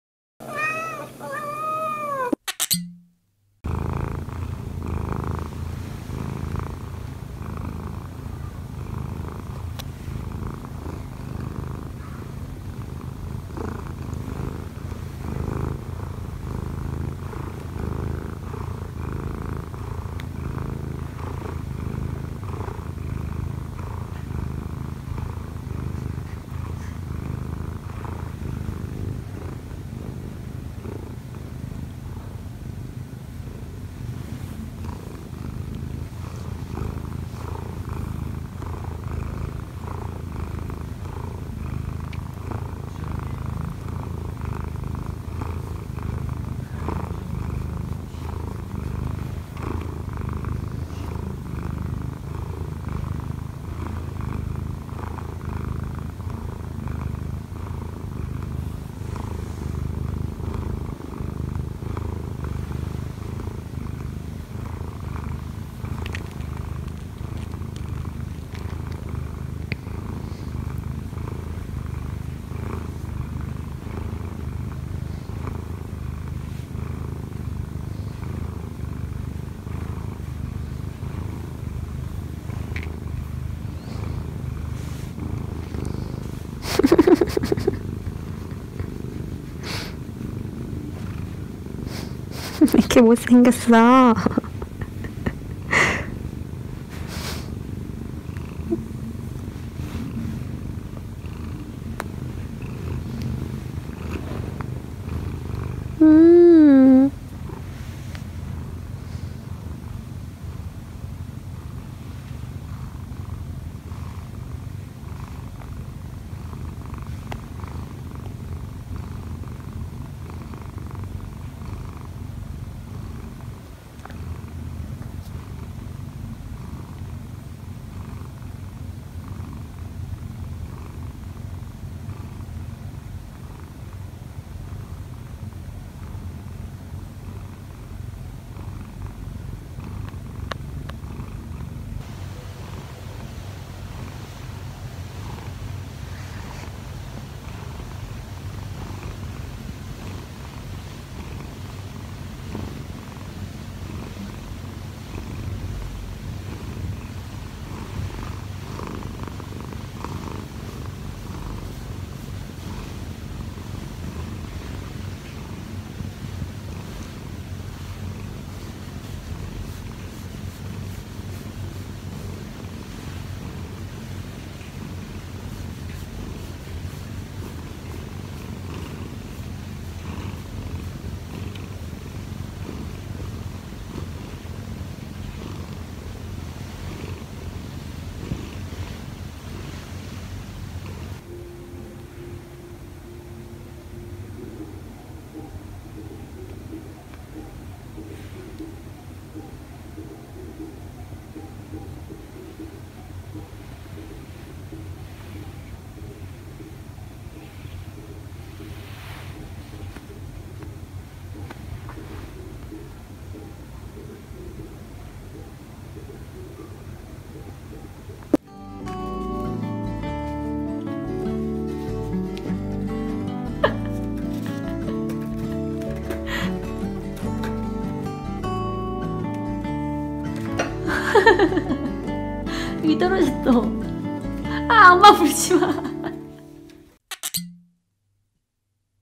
엄마 위에서 꾹꾹이 골골이 하는 꼬부기 - 1살+4개월 오드아이 먼치킨 고양이 Munchkin Cat Gato マンチカンねこ 短足猫
꼬부기는 엄마를 너무 좋아해요
엄마가 늦게 일어나면 엄마한테 올라가서 폭풍 꾹꾹이 골골이!
아내를 제일 기분 좋게 깨우는 방법은 꼬부기를 올려두는거에요